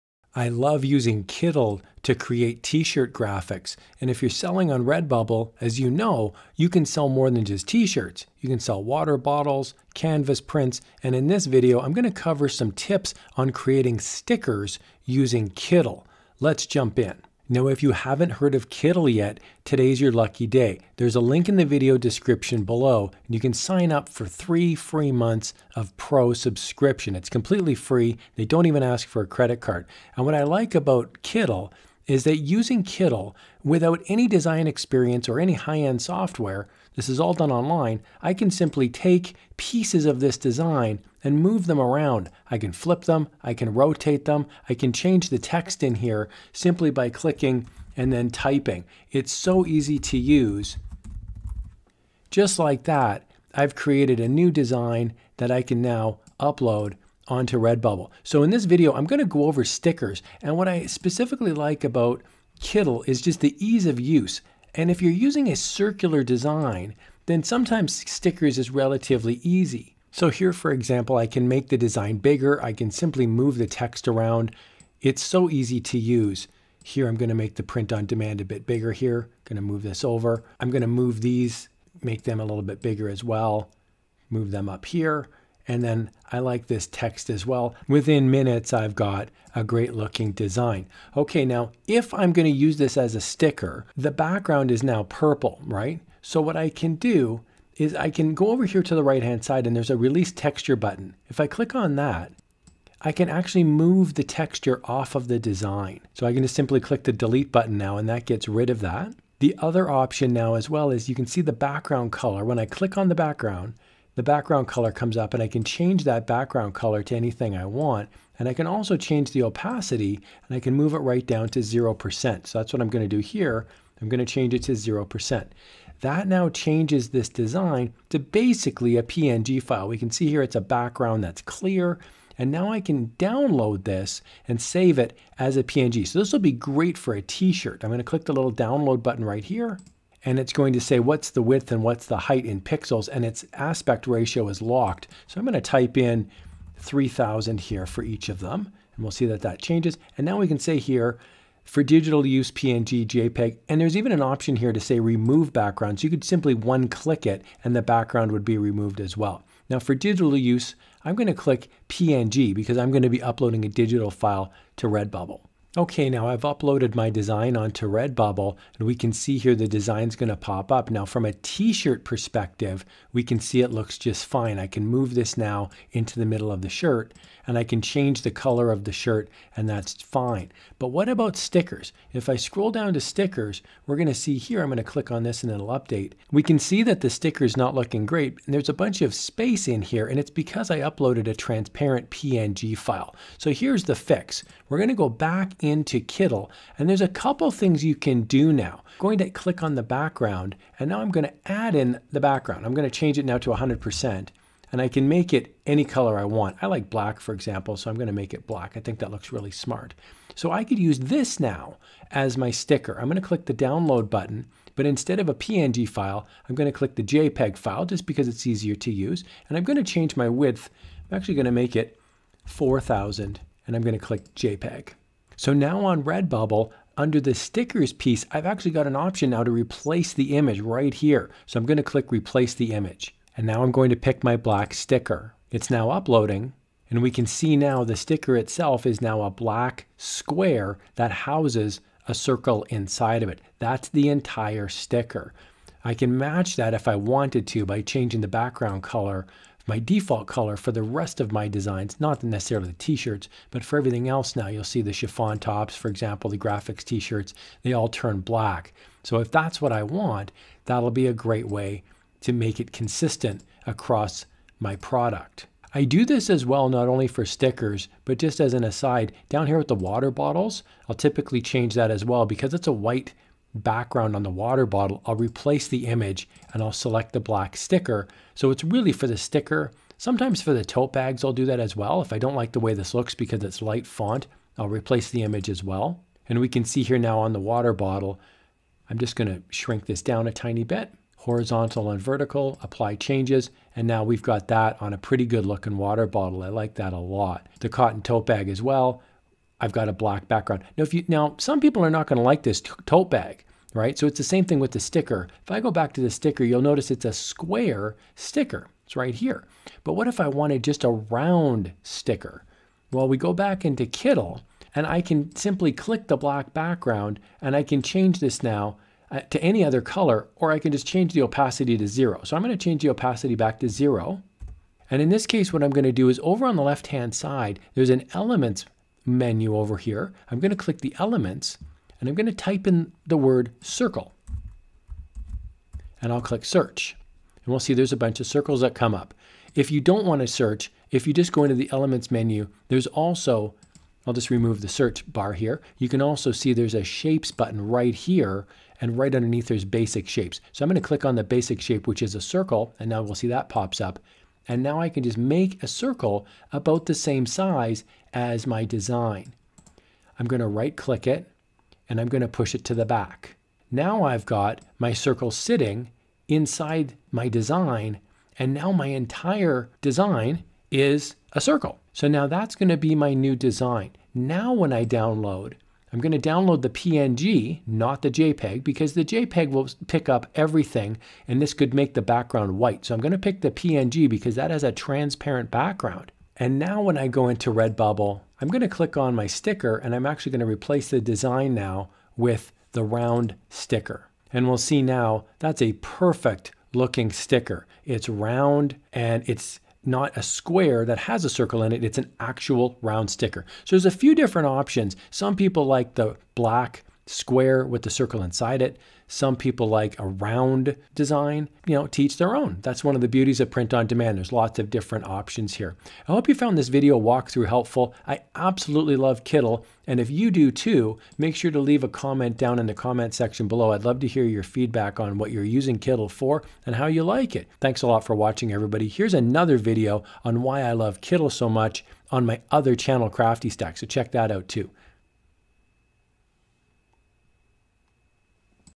Use KITTL To Create STICKERS on REDBUBBLE
Kittl is a great website that enables designers to create amazing graphics in seconds. In this video I cover STICKERS (which can be different than T-SHIRTS) on Redbubble. There are a couple of tips and tricks you can use to make your stickers stand out and make sales!!

Kittl works right in your web browser (no software is needed). It is complete with fonts, an illustration library high-quality templates and much more without the need for expensive software (and hours to learn them). Just click, add, edit and CREATE!

Timestamps:
00:00 What is Kittl?
00:17 Templates
01:33 Backgrounds
03:06 Sticker Method #1
05:00 Sticker Method #2